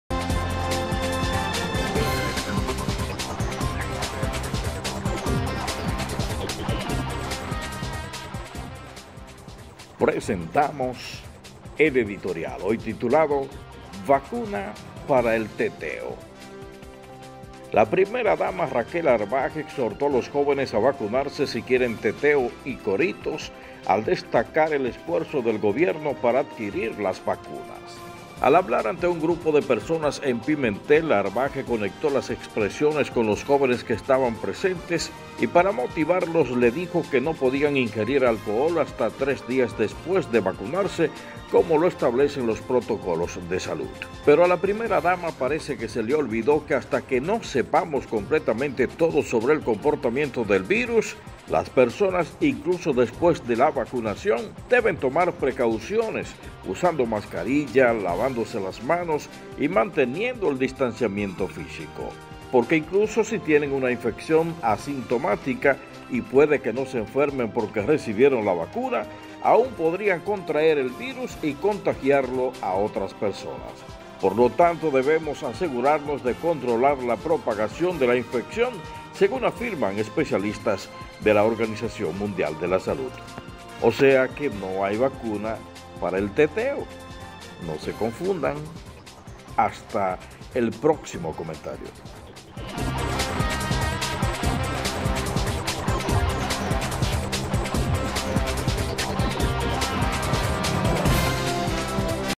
Editorial de NTN: "Vacuna para el teteo"
La Primera Dama Raquel Arbaje exhortó a los jóvenes a vacunarse si quieren “teteo” y “coritos”, al destacar el esfuerzo del Gobierno para adquirir las vacunas.

Al hablar ante un grupo de personas en Pimentel, Arbaje conectó las expresiones con los jóvenes que estaban presentes, y para motivarlos les dijo que no podían ingerir alcohol hasta tres días después de vacunarse como lo establecen los protocolos de salud.

Pero a la primera dama parece que se le olvidó que hasta que no sepamos completamente todo sobre el comportamiento del virus, las personas, incluso después de la vacunación, deben tomar precauciones, usando mascarilla, lavandose las manos, y manteniendo el distanciamiento físico, porque incluso si tienen una infección asintomática y puede que no se enfermen porque recibieron la vacuna, aún podrían contraer el virus y contagiarlo a otras personas.

Por tanto, debemos asegurarnos de controlar la propagación de la infección”, según afirman especialistas de la OMS.

O sea que no hay VACUNA PARA EL TETEO.